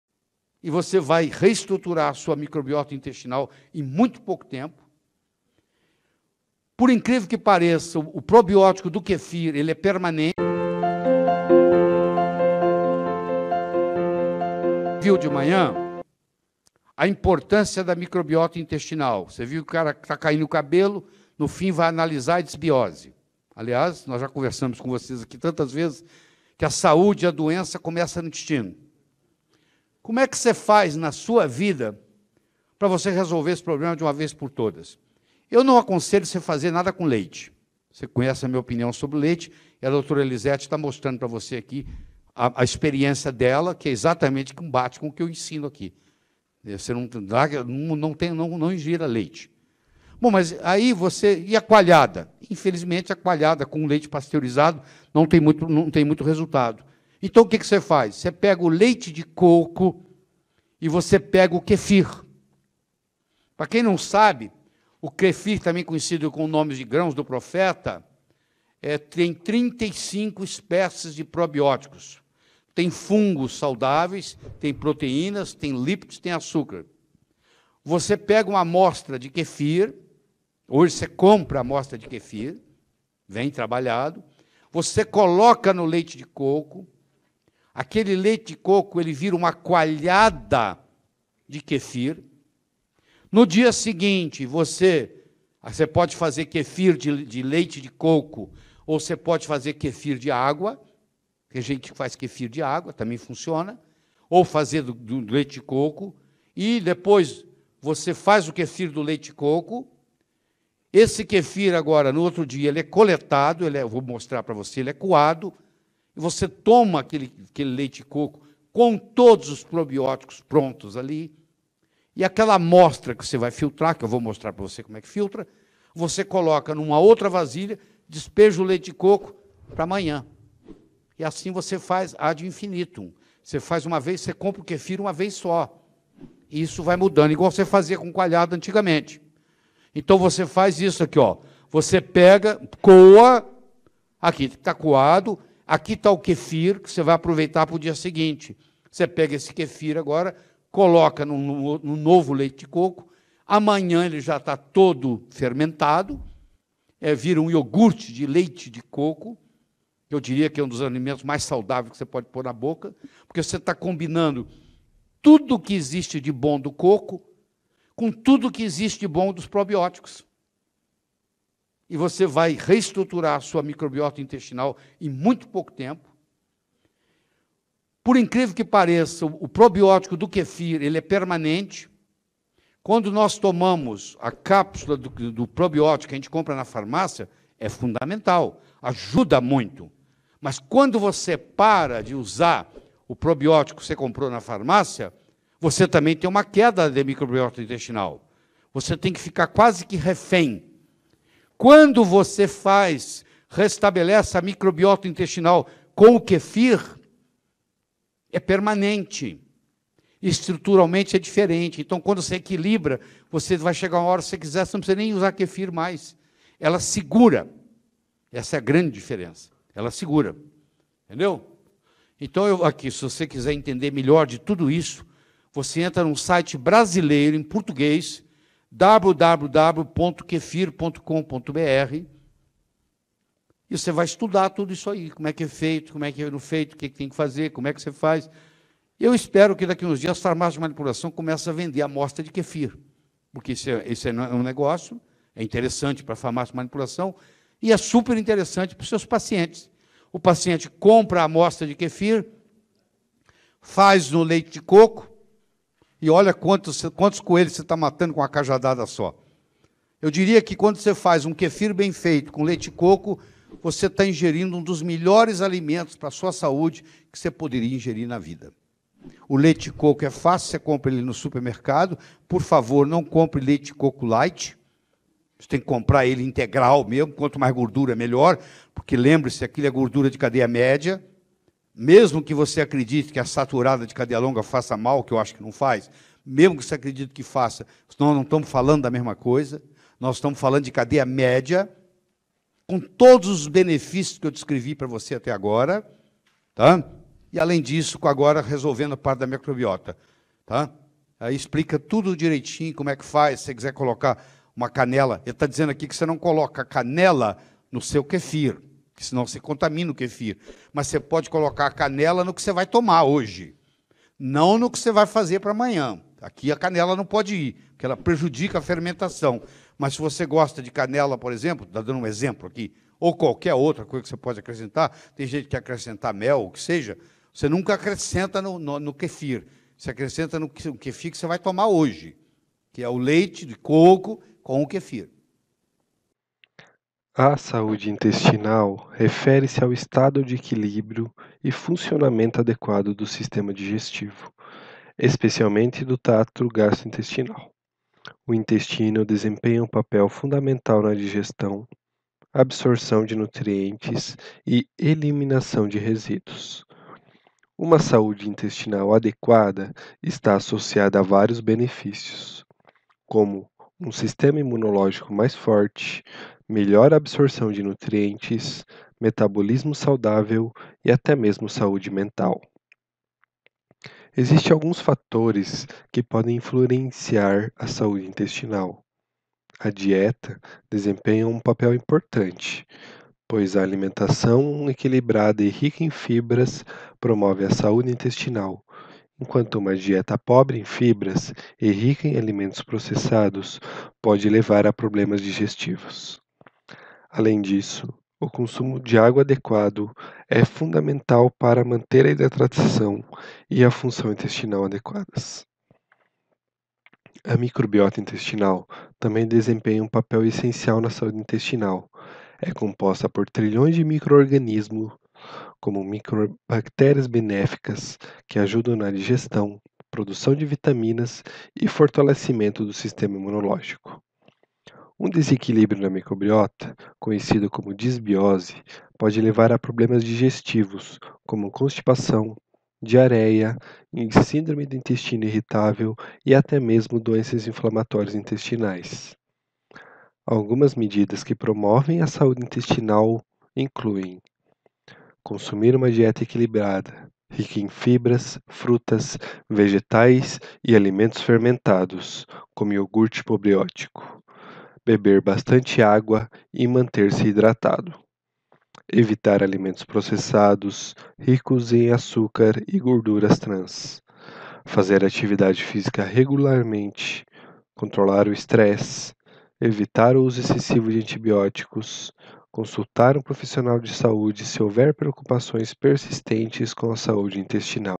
Como recuperar sua microbiota intestinal ‖ Dr. Lair Ribeiro
O kefir é uma bebida que melhora a flora intestinal, auxilia a imunidade e melhora o trânsito intestinal, isso porque é constituído por bactérias e leveduras probióticas, ou seja, que promovem a saúde geral do organismo.

As bactérias do kefir podem ser cultivadas em casa com segurança e a produção da bebida é fácil e se assemelha à produção de iogurte natural. Existem dois tipos de kefir, o de leite e o de água, que contém as mesmas bactérias e leveduras, porém adaptadas a ambientes distintos. Além disso, esses dois tipos de kefir podem ser diferenciados de acordo com as enzimas presentes em sua composição.

MELHOR JEITO DE LIBERAR SEU INTESTINO